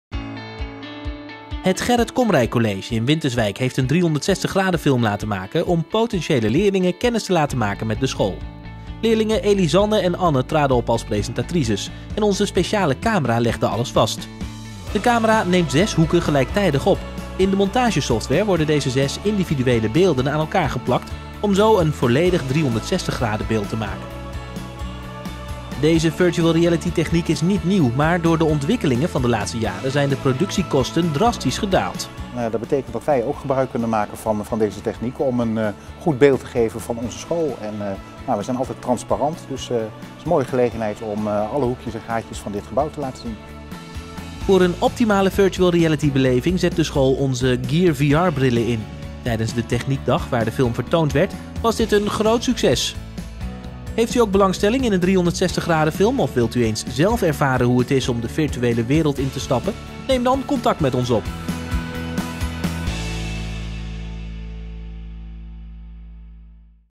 Virtual Reality 'Behind the Scenes'
Met een 360 graden film wil het Gerrit Komrij College in Winterswijk potentiële leerlingen kennis laten maken met de school. Wij maakten de virtual reality film. In deze normale film geven we een kijkje achter de schermen.